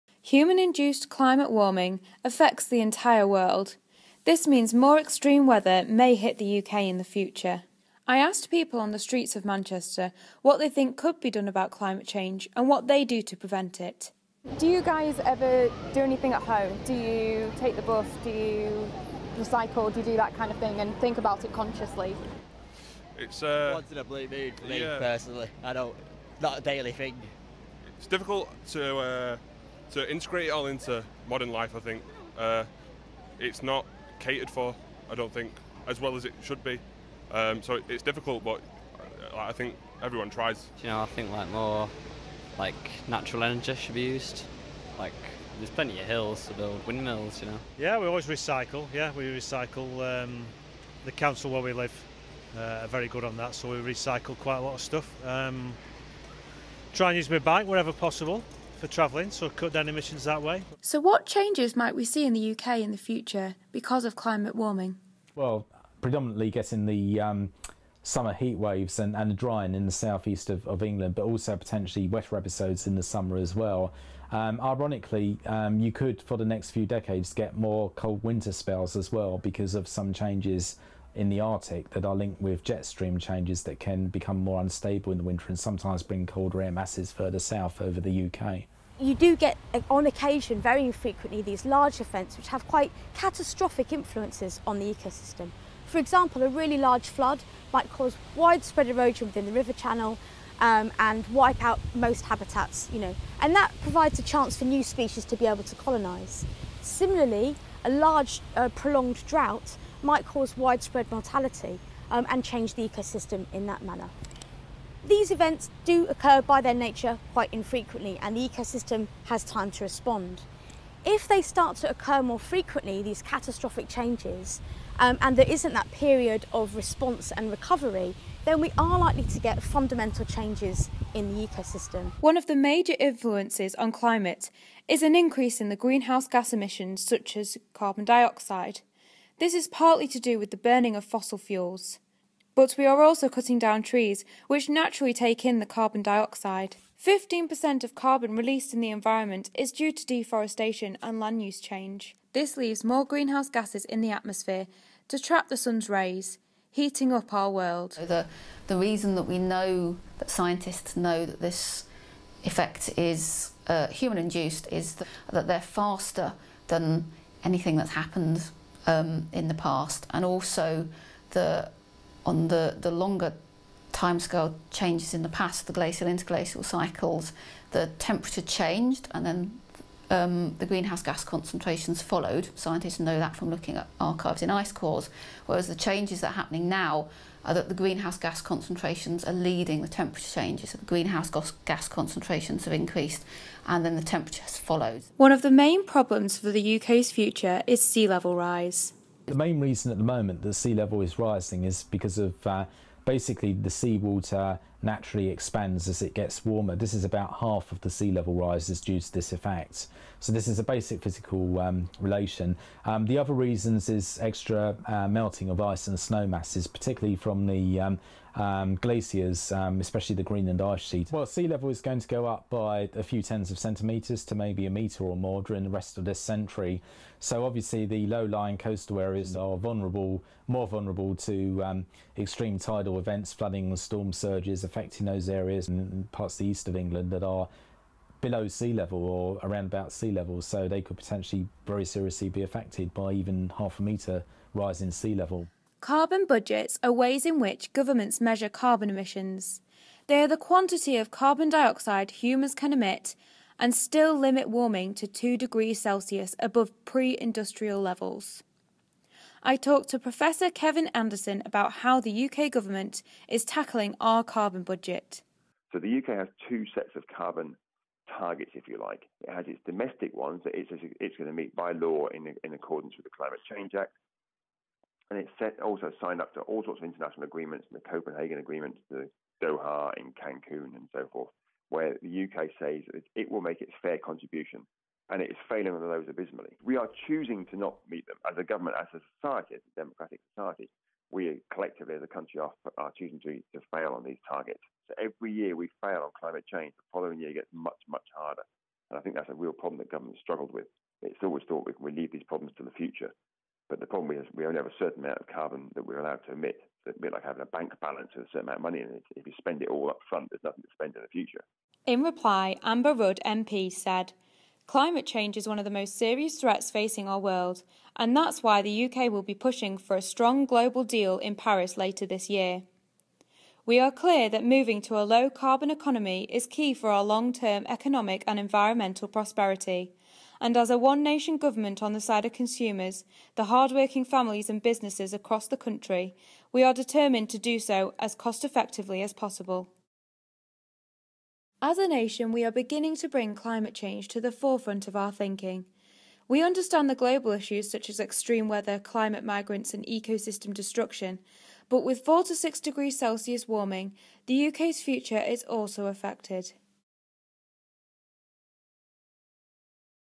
The Future
The UK's Extreme Weather:
Does the average person care about climate change? And what do government policies on climate change mean for our future? Lois Martin checks out what the UK's future looks like at our current rate of climate change.